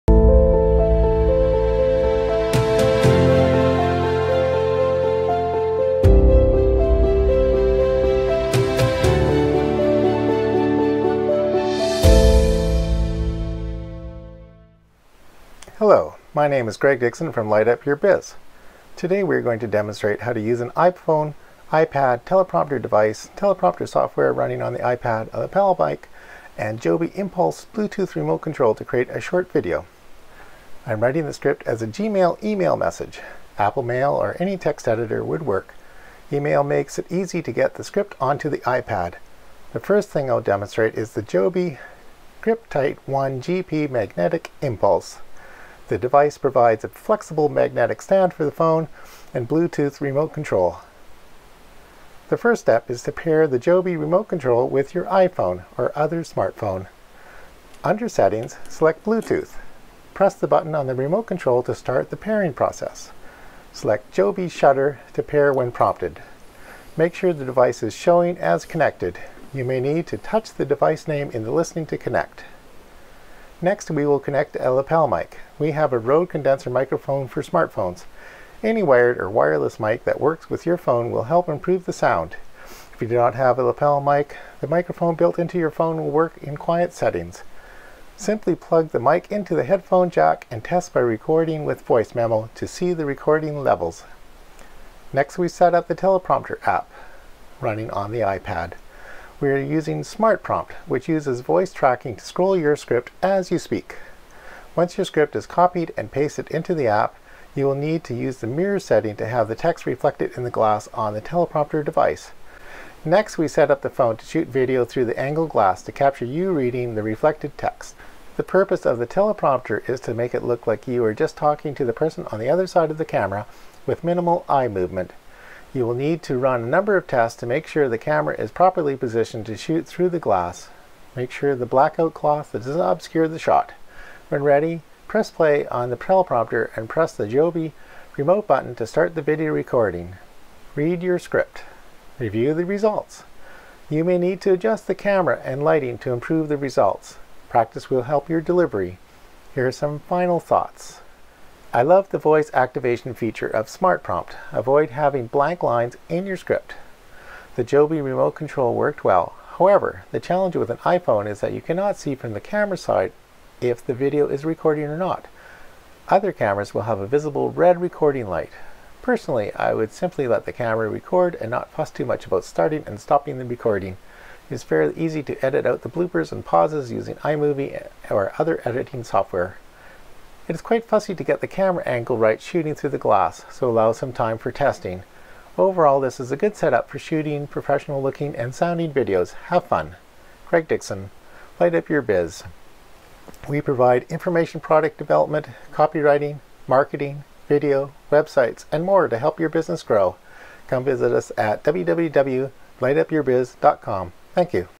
How to Use An iPhone with a Teleprompter and the PromptSmart App
My name is Greg Dixon from Light Up Your Biz.

Today we are going to demonstrate how to use an iPhone, iPad, teleprompter device, teleprompter software running on the iPad, a lapel mic, and a Joby Impulse bluetooth remote control to create a short video.

I am writing the script as a gmail email message.  Apple Mail or any text editor would work.  Email makes it easy to get the script on to the iPad.

The first thing I will demonstrate is the JOBY GripTight ONE GP Magnetic Impulse.

The device provides a flexible magnetic stand for the phone and bluetooth remote control.

The first step is to pair the Joby remote button with your iPhone or other smartphone.

Under settings select Bluetooth.

Press the button on the remote control to start the pairing process.

Select JOBY Shutter to pair when prompted.

Make sure the device is showing as Connected.  You may need to touch the device name in the listing to connect.

Next we connect a lapel mic.

We have a Rode Condenser Microphone for Smartphones.  Any wired or wireless mic that works with your phone will help improve the sound.

If you do not have a lapel mic, the microphone built in to your phone will work in quiet settings.

Simply plug the mic into the headphone jack and test by recording with Voice Memo to see the recording levels.

Next we set up the teleprompter app running on the iPad.

We are using PromptSmart, which uses voice tracking to scroll your script as you speak.

Once your script is copied and pasted into the App, you will need to use the mirror setting to have the text reflected in the glass of the teleprompter device.

Next we set up the phone to shoot video through the angled glass to capture you reading the reflected text.

The purpose of the teleprompter is to make it look like you are just talking to the person on the other side of the camera with minimal eye movement.

You will need to run a number of tests to make sure the camera is properly positioned to shoot through the glass.

Make sure the blackout cloth does not obscure the shot.

When ready, press play on the teleprompter and press the Joby remote button to start the video recording.

Read your script.

Review the results.  You may need to adjust the camera and lighting to improve the results.

Practice will help your delivery.

Here are some final thoughts.

I loved the voice activation feature of SmartPrompt.

The JOBY Remote control worked well.  However, the challenge with an iPhone is that you cannot see from the camera side if the video is recording or not.  Other cameras will have a visible red recording light.

Personally I would simply let the camera record and not fuss too much about starting and stopping the recording. It is fairly easy to edit out the bloopers and pauses using iMovie or other editing software.

It is quite fussy to get the camera angle right shooting through the glass, so allow some time for testing.

Overall this is a good setup for shooting professional looking and sounding videos.

Have Fun!

G​reg Dixon

Light Up Your Biz
Light Up Your Life
Information product development, copywriting, marketing, video, websites, and more to help your business grow.